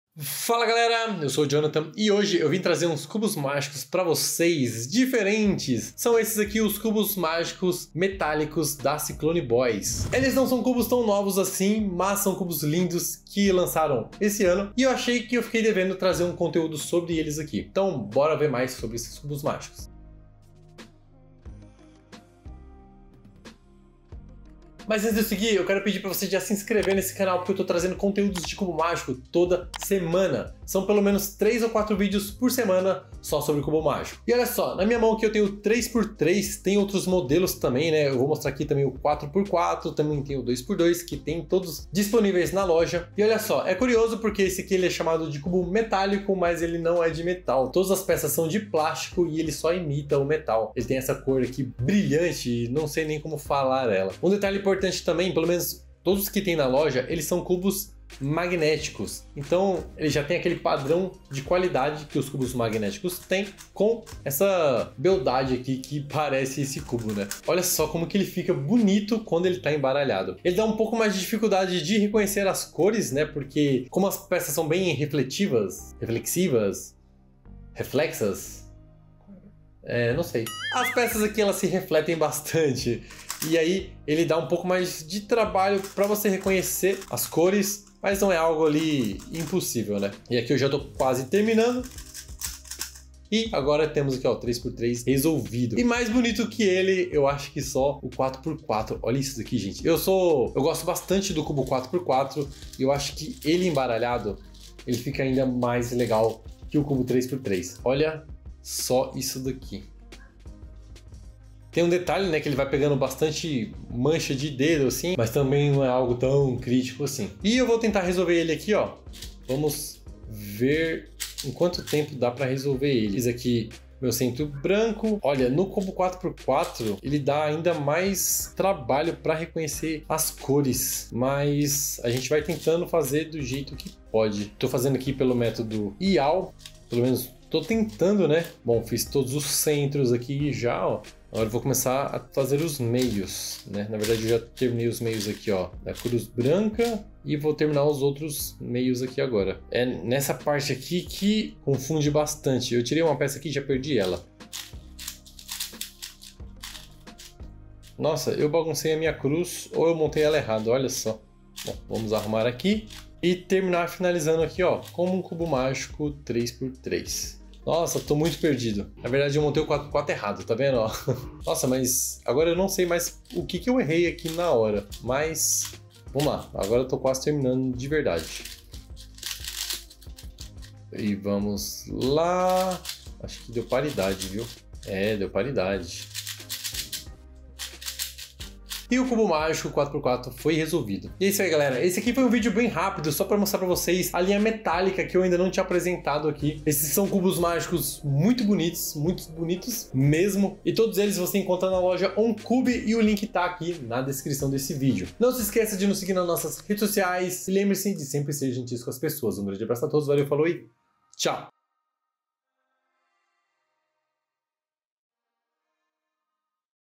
CUBOS METÁLICOS, OS MAIS BONITOS QUE JÁ VI
Fala galera, nesse vídeo te mostro os cubos mágicos metálicos da marca Cyclone Boys.
São lindos modelos que todo cubista gostaria de ter na coleção.

Não se esqueça de curtir e comentar :-D

Quer comprar esse cubo?

QUER APRENDER A RESOLVER O CUBO MÁGICO?